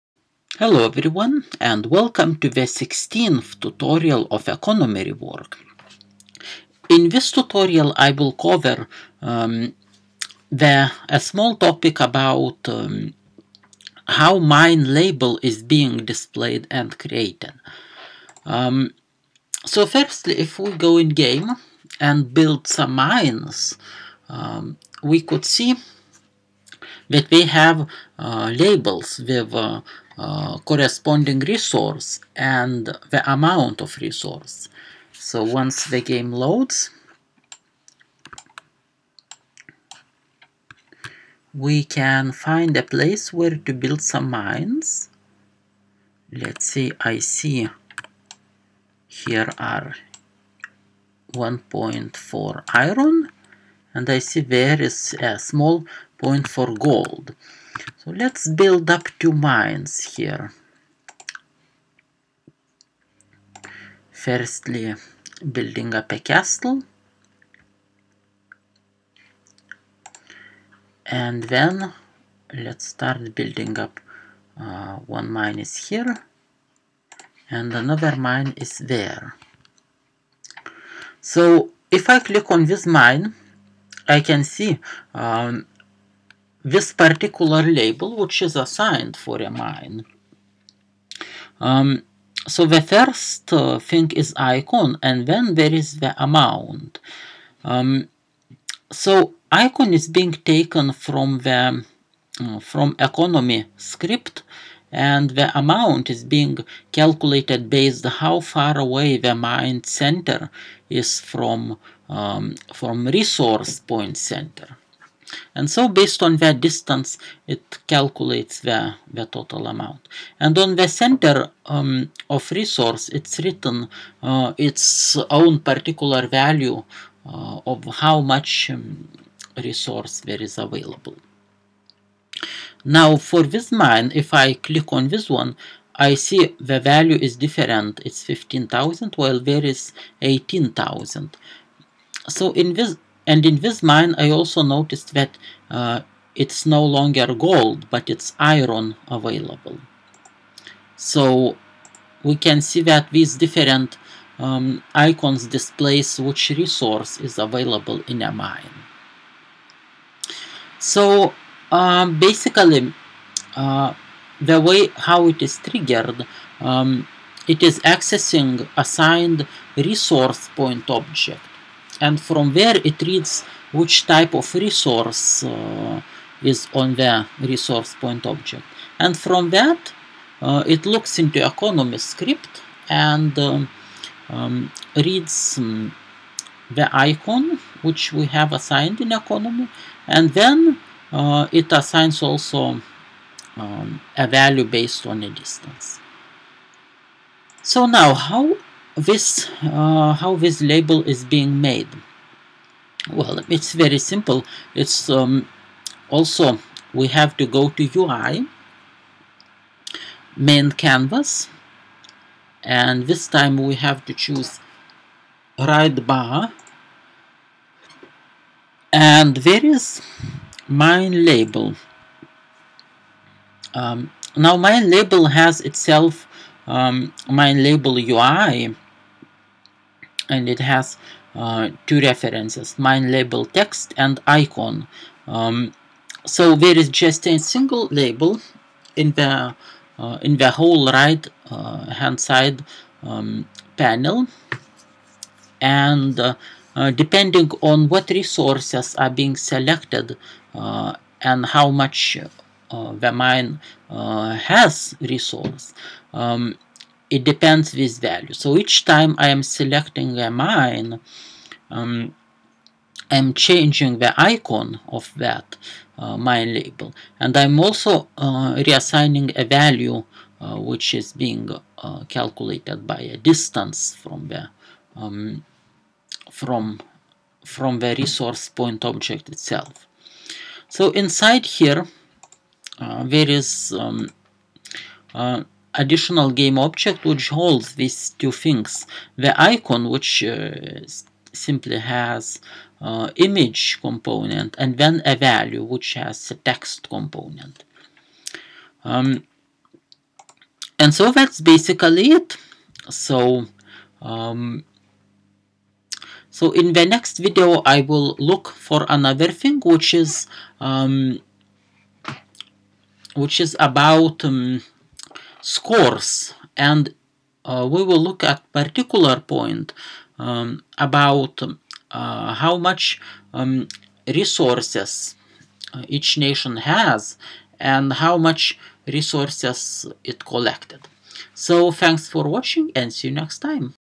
[Unity3d] uRTS: Economy rework: Mine label UI (Pt16)
This tutorial is based on uRTS Toolkit v2017.2. Earlier and later versions may have different appearance and different features.

First game powered by uRTS is "Tales of Eightrivers", which is running on Facebook: